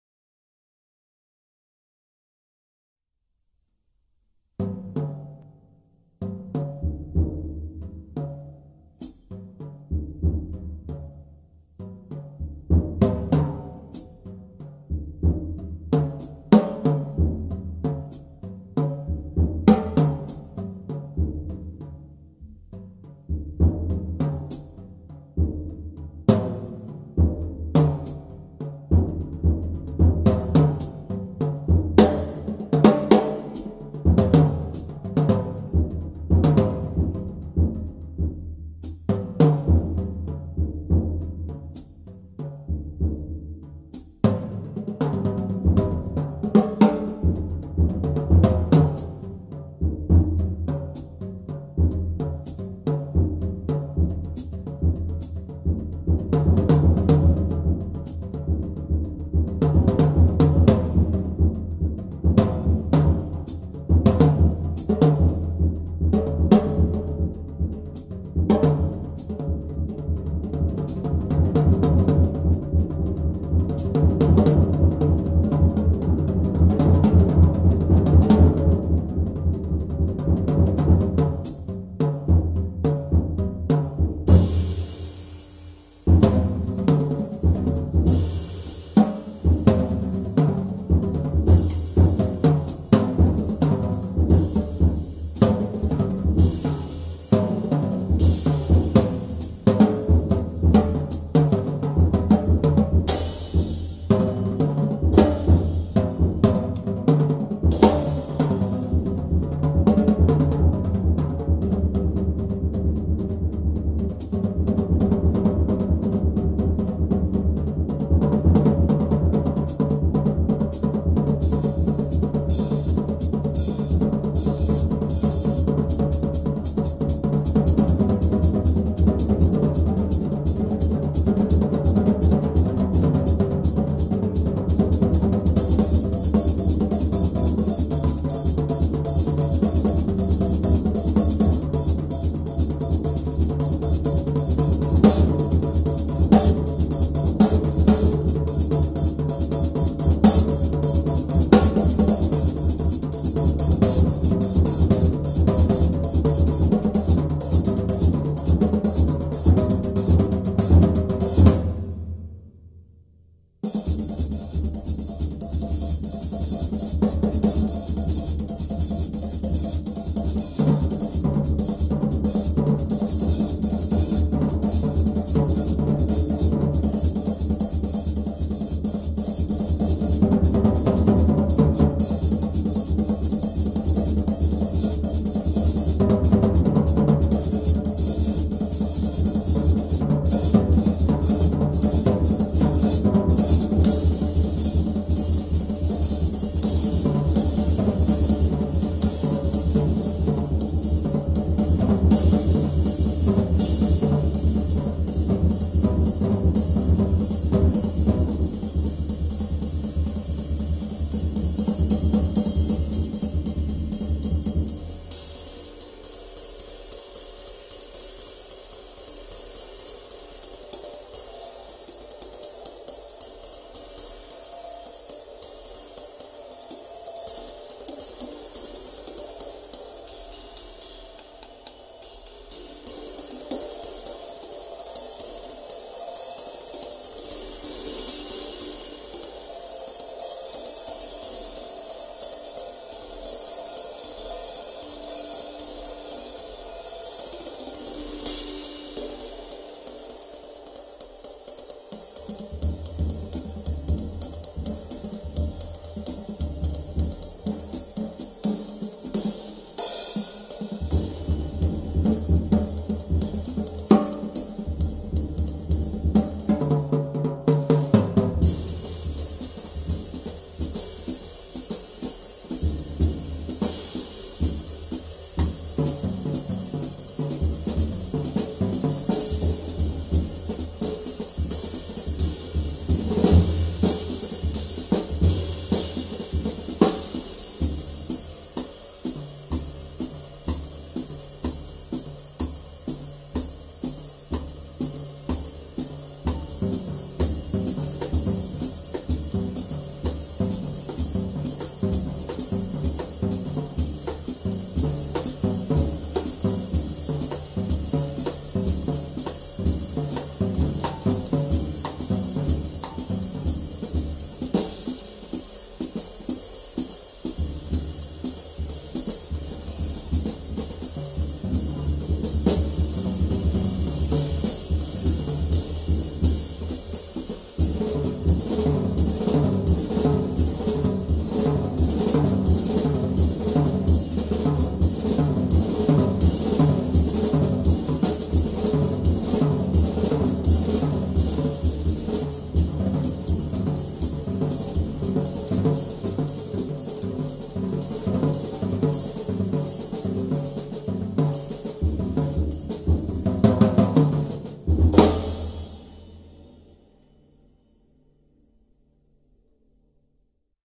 FREE FORM JAZZ DRUMS SOLO
Just testing my AKG MICS in my  new drum studio